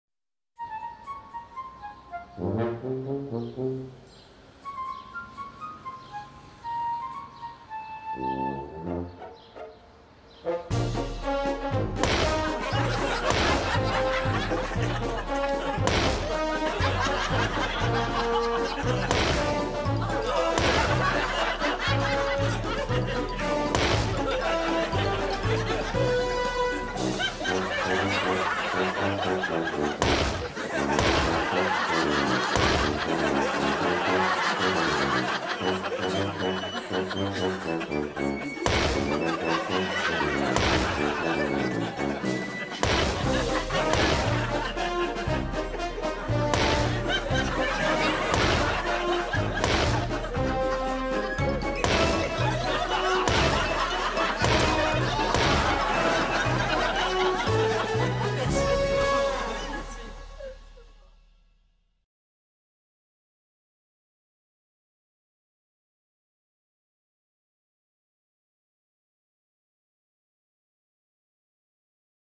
EXTREME EXPLODING BACKPACK PRANK
Maybe not ALL CAPS EXTREME but she still gets a decent BANG out of that backpack. What could she be carrying in that backpack of hers?